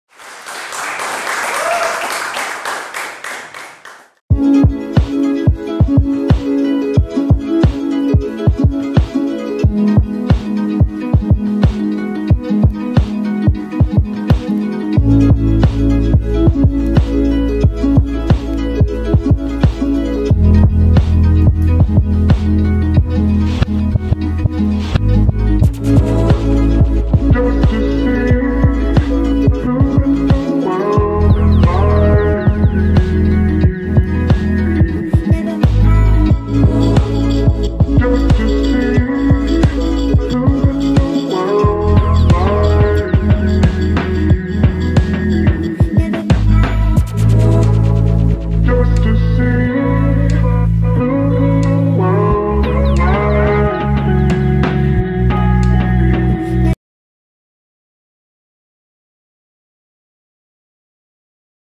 Open Mic Night - Episode 3 at 641
Video FILMED & EDITED : Shanthosh Ravikumar

Spotlight on artist - is an open mic night event where the budding and talented artist of Coimbatore are allowed to perform without any charges and seize the crowd with their performance. Includes musicians, singers, stand up comedian, mimicry, poets, etc.